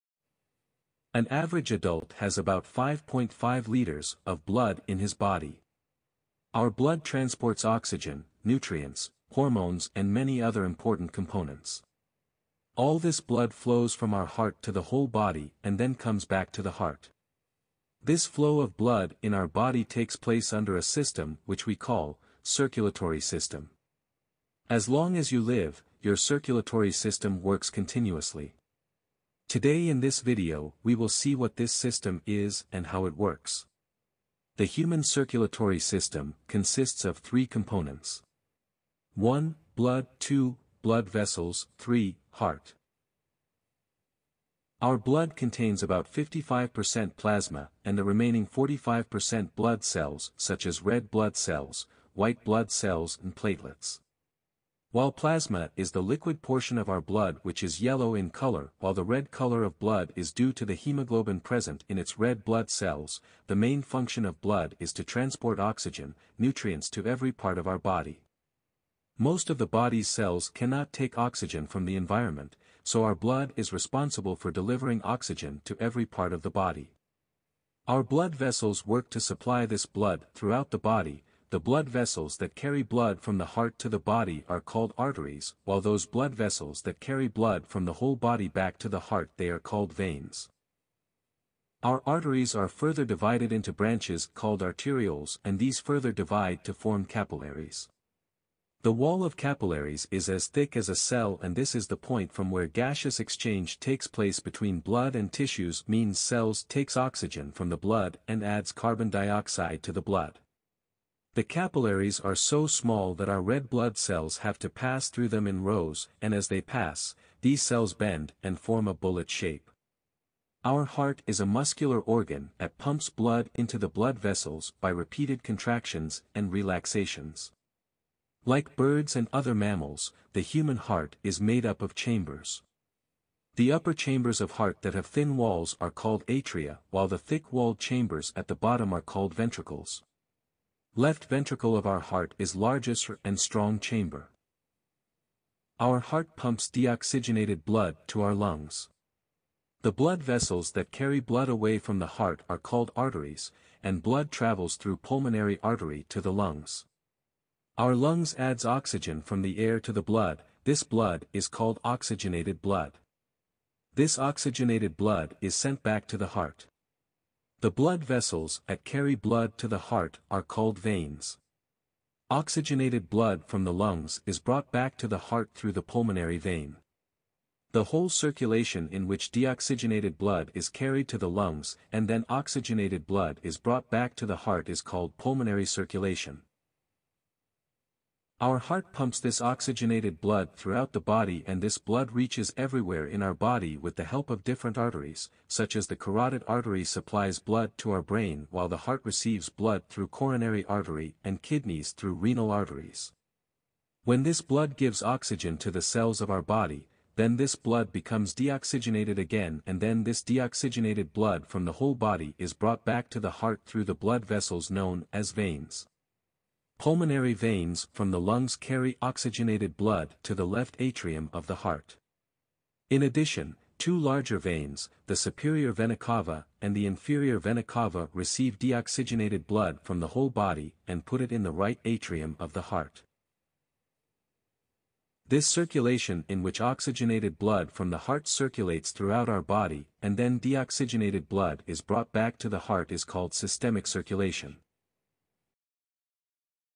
Human Blood Circulatory System - Explained
An average adult has about 5.5 liters of blood in his body.  Our blood transports oxygen, nutrients, hormones and many other important components.  All this blood flows from our heart to the whole body and then comes back to the heart.
 This flow of blood in our body takes place  a system which we call, Circulatory system.  As long as you live, your circulatory system works continuously.  Today in this video we will see what this system is and how it works.